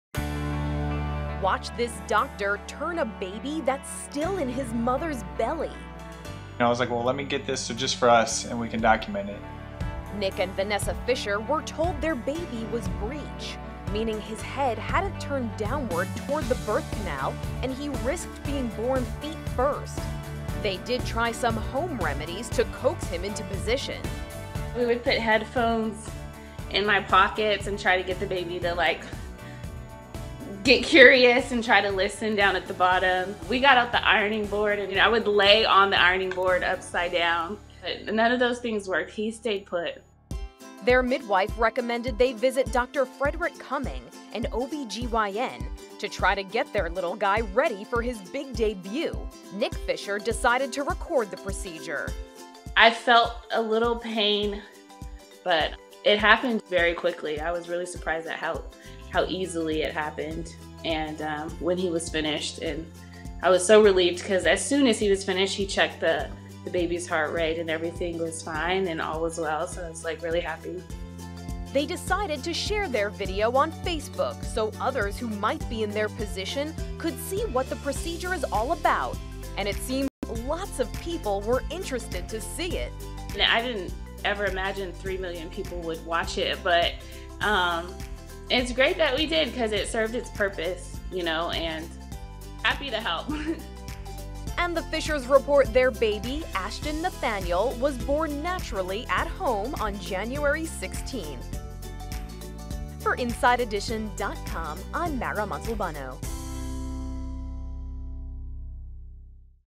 Doctor Turns Breech Baby Still in Mother's Belly
Watch this doctor turn a baby that's still in his mother's belly. Nick and Vanessa Fisher were told their baby was breech, meaning his head hadn't turned downward toward the birth canal and he risked being born feet first. At first they tried home remedies to coax him into position, but when that didn't work, their midwife recommend they visit an OB-GYN to try to get their little guy ready for his big debut. InsideEdition.com's Mara Montalbano has more.